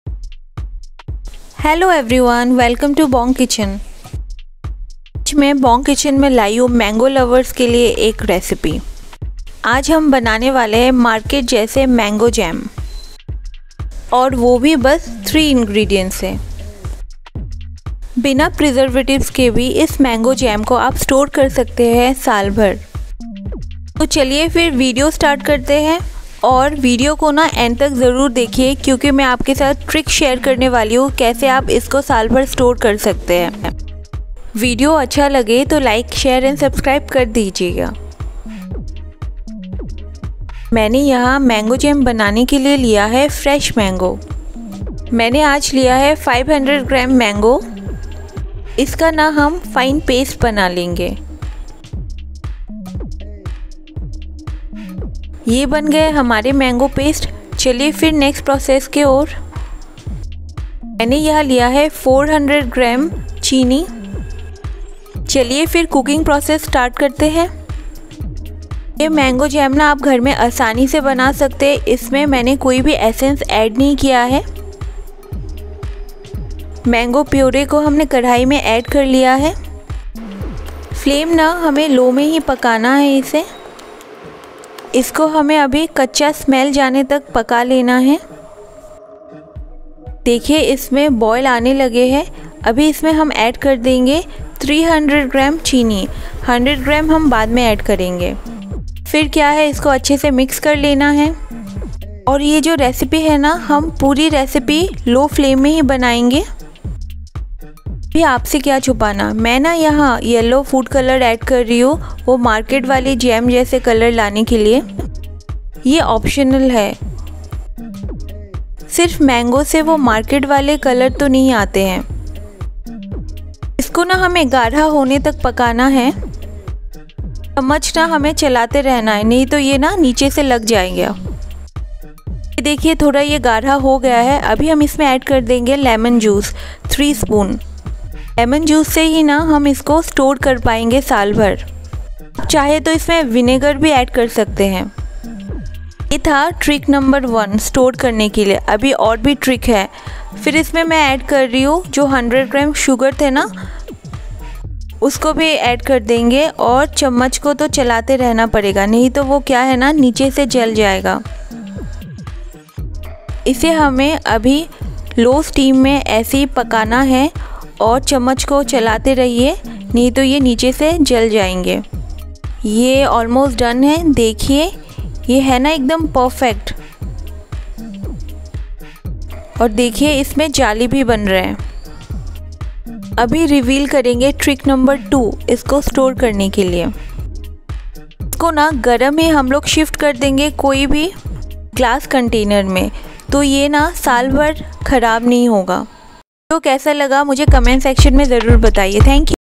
Easy and Quick Mango Jam at home with only 3 ingredients | No preservative | Mango recipe
Do try at your home :)
 Shower some Love

Thank you lovelies😊